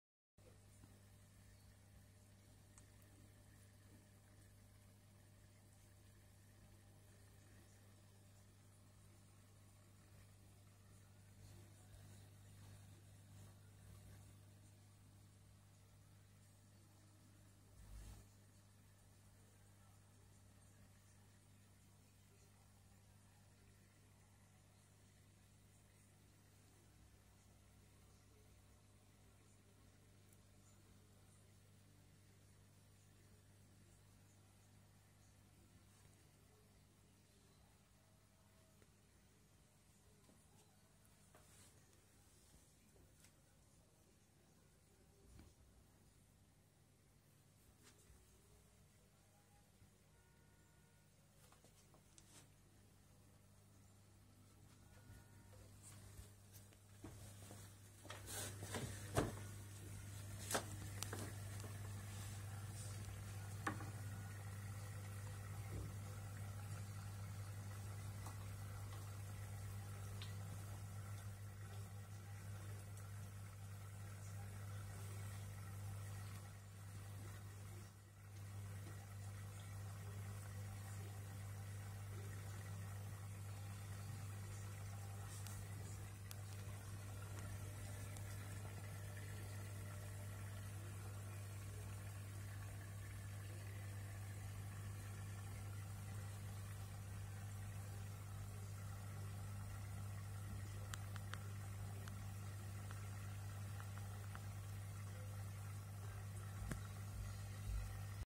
Vétó (Betta) new home
Our Betta (Vétó) and his new home.
And the another fishes.